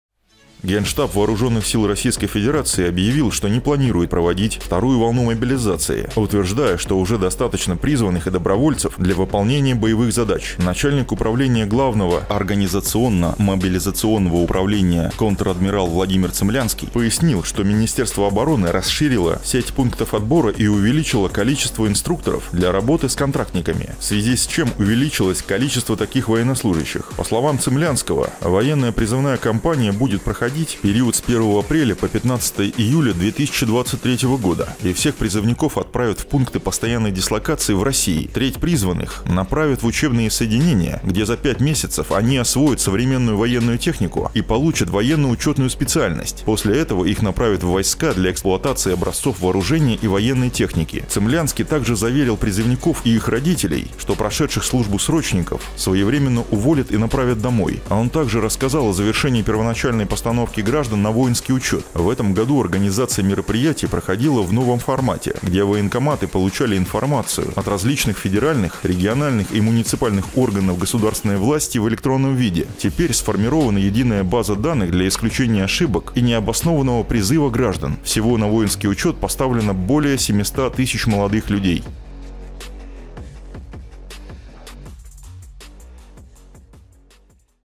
Вторая волна мобилизации: заявление Генштаба ВС РФ
Начальник управления Главного организационно-мобилизационного управления ГШ ВС РФ контр-адмирал Владимир Цимлянский сделал важное заявление (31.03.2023 г.)